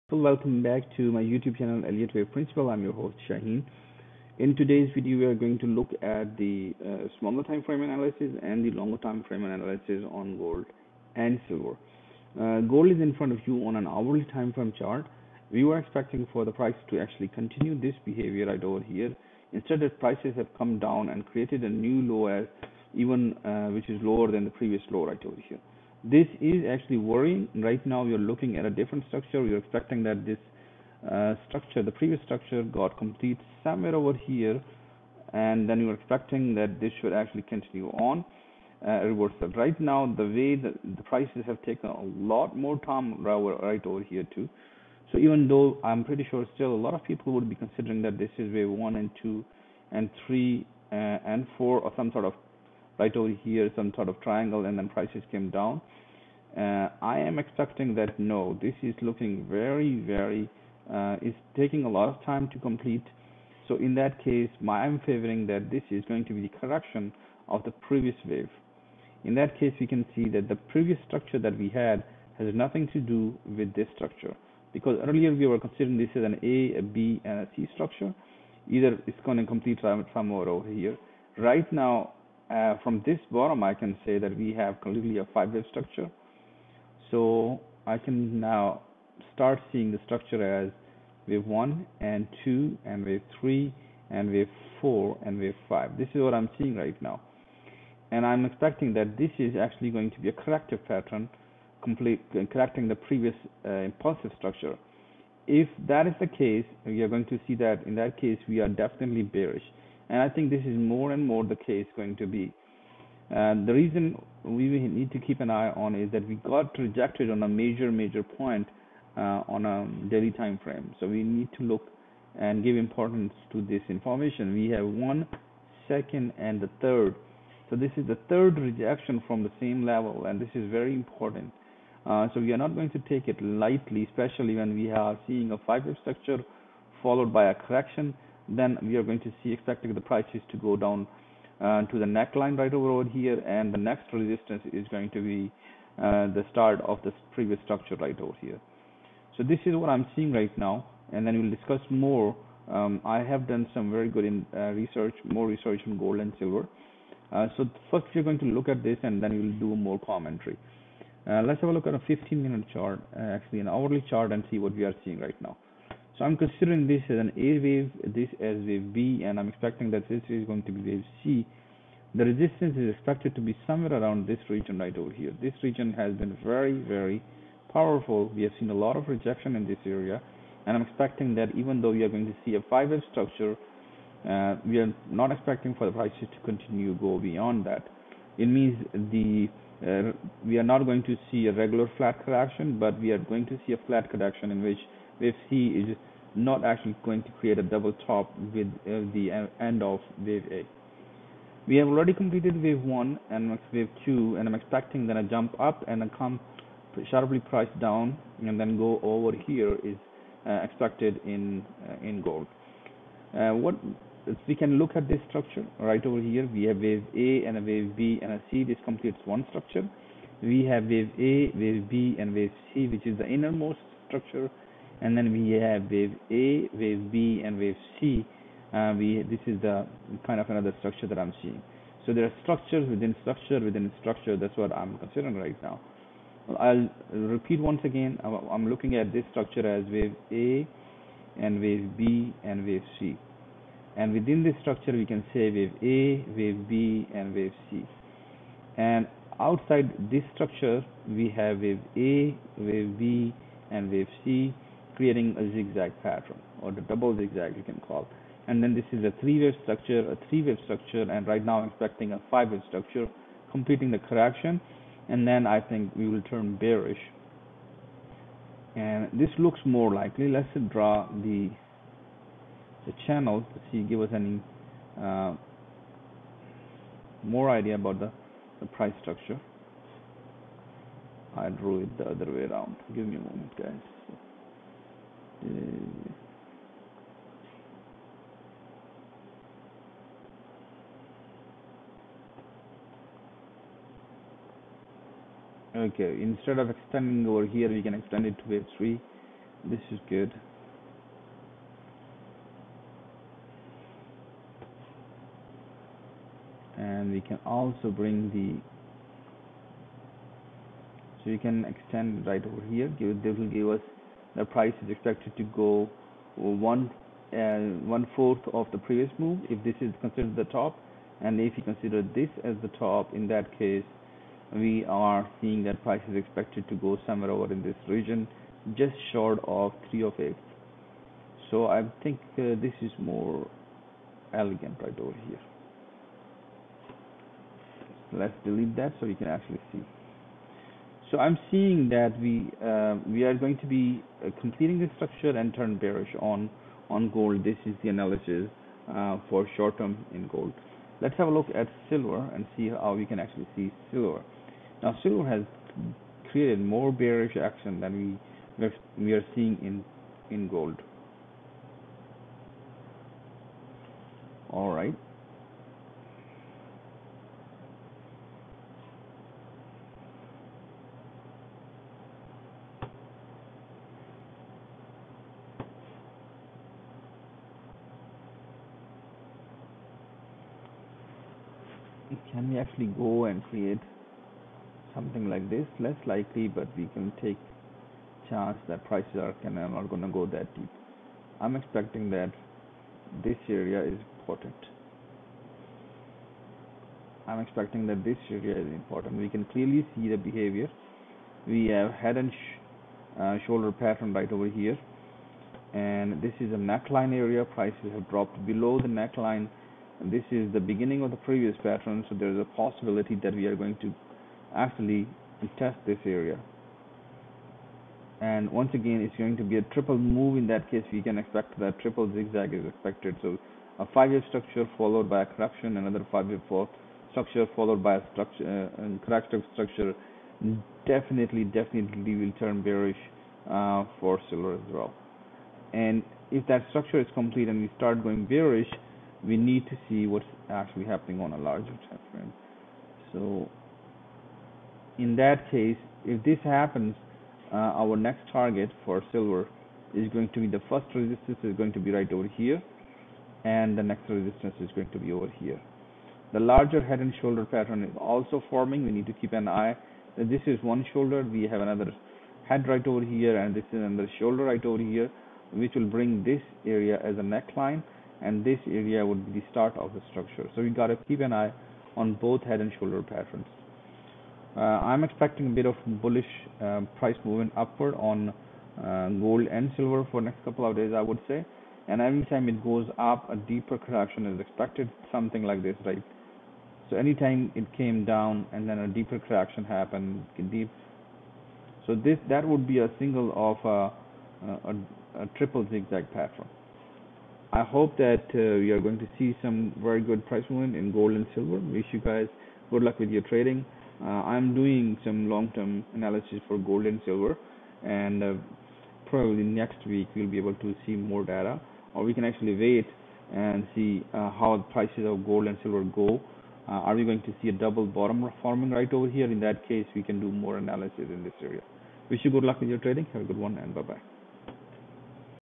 Elliott wave analysis of GOLD and SILVER | June 26 2023
Gold is showing interesting behavior, check short term and long term analysis for Gold and SIlver.